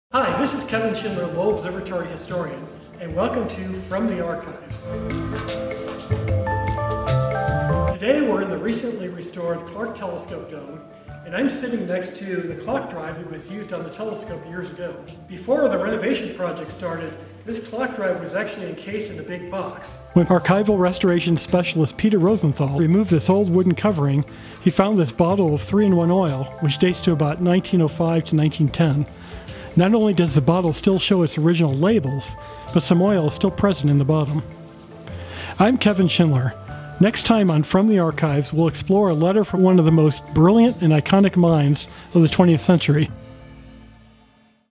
3-in-1 Oil Bottle
Did you know there was a rare, historic artifact discovered during the Clark Telescope Renovation? Lowell Historian, Kevin Schindler has the details.